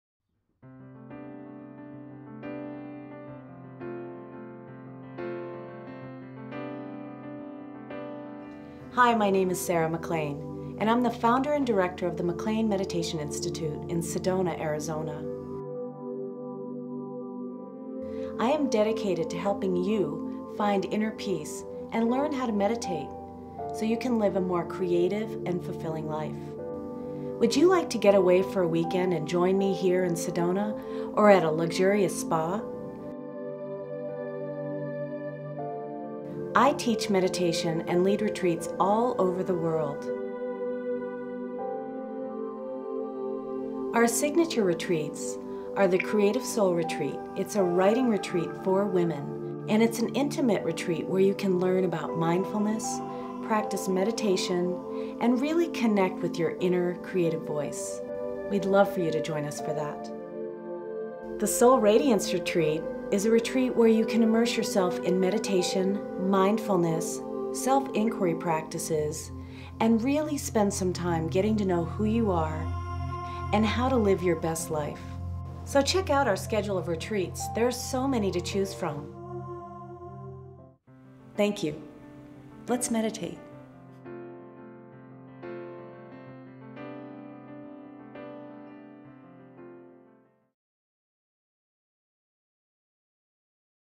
McLean Meditation Institute offers meditation and creative retreats with Sarah McLean
Sarah McLean, founder of the McLean Meditation Institute, leads meditation and creative retreats in Sedona, Arizona and at spa destinations around the world.

Creative Soul Retreat™ is a women's meditation and writing workshop. This retreat offers deep meditation practices, where you learn to listen and trust your own voice. This program is open to all levels with no meditation or writing experience necessary.

Soul Radiance™ is a weekend retreat to reconnect with your spirit, awaken your full potential, and strengthen your soul connection. This essential journey includes mindful movement and self-discovery exercises as well as silent meditation.